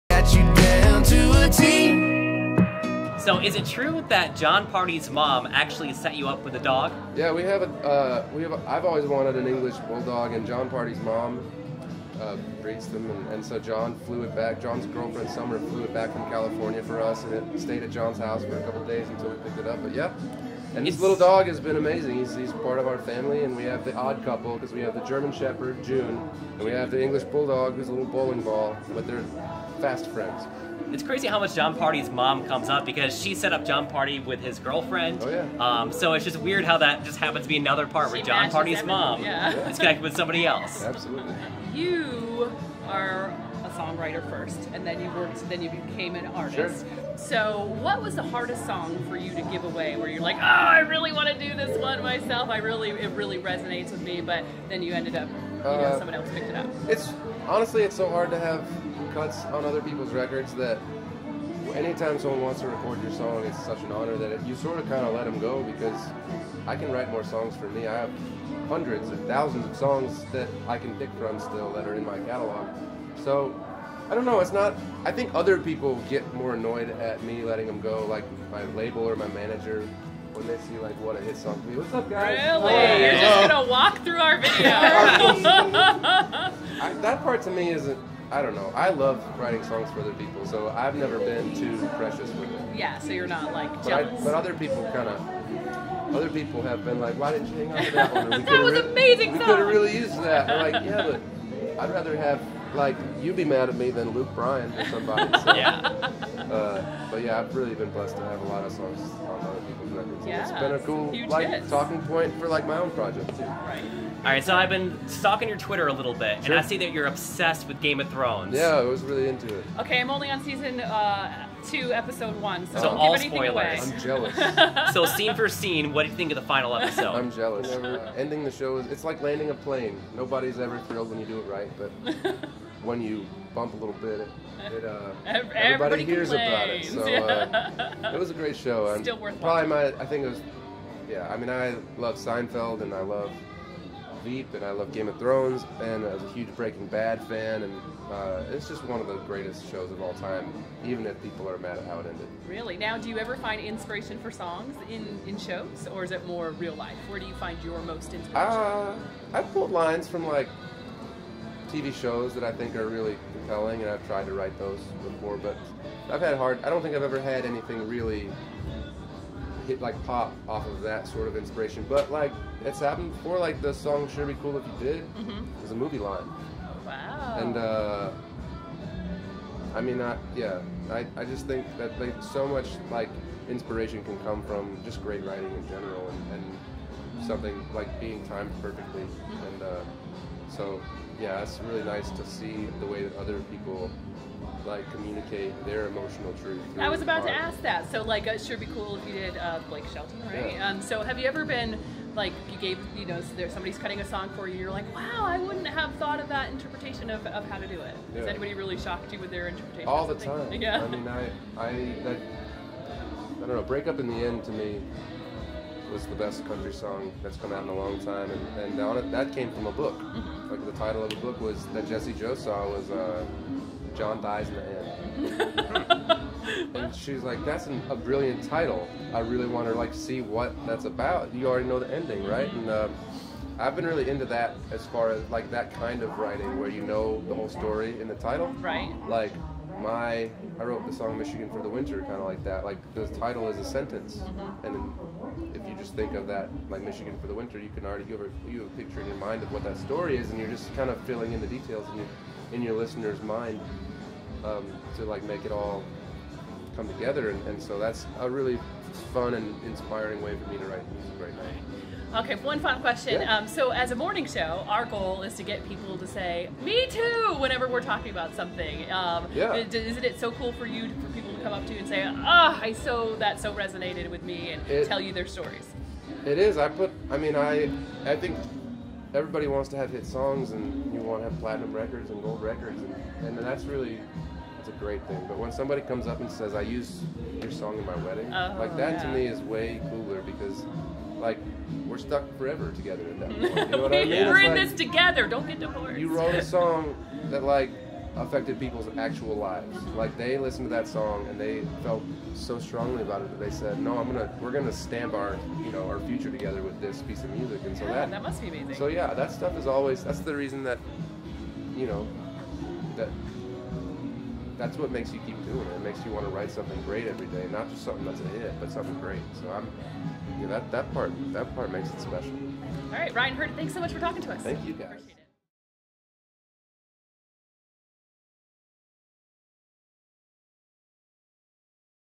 Ryan Hurd on his dog Pancake, Game of Thrones, combining movies with songwriting & more!
Ryan Hurd talked to KC & Kelly before his show at Saratoga Casino Hotel about his dog Pancakes he got from Jon Pardi's mom, Game of Thrones, combining movies with songwriting and more!